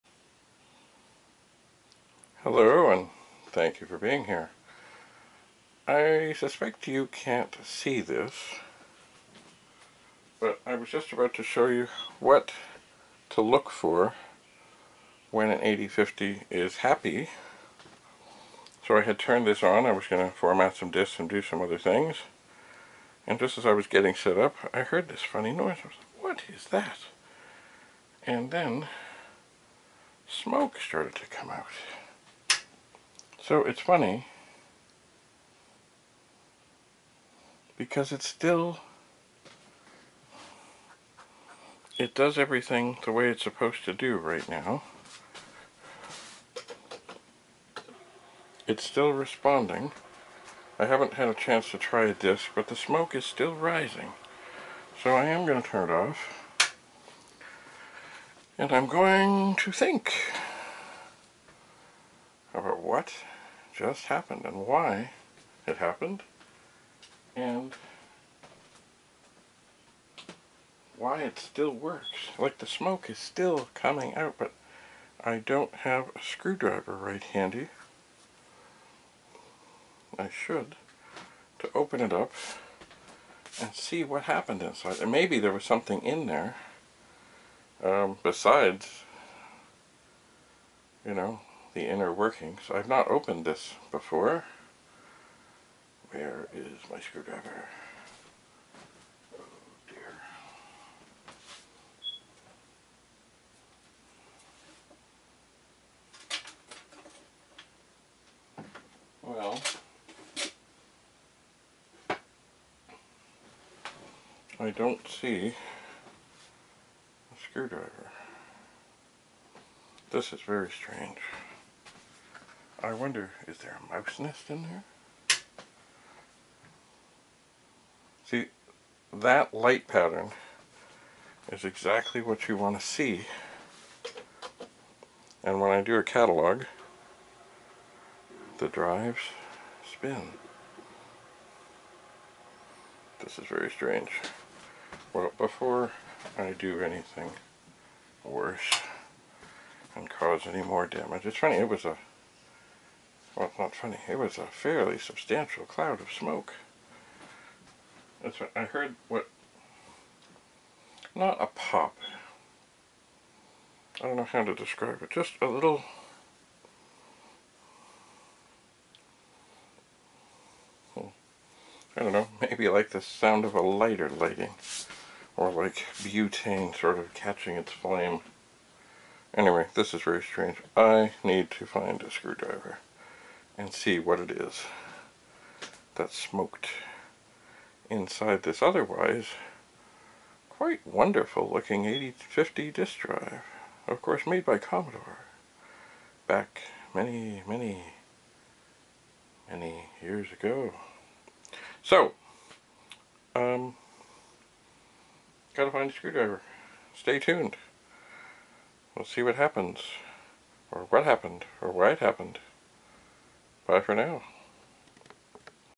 Commodore - Episode 472 - SMOKE Erupts From An 8050 Dual Disk Drive - Dave Bradley - PET CBM 8-Bit
I was getting ready to shoot this 8050 disk drive to show what the LED's do on powerup and when the drive is operating, when I heard a little noise...even though I was not accessing the disk drive and then smoke billowed from the air vents, and I reached for the power switch!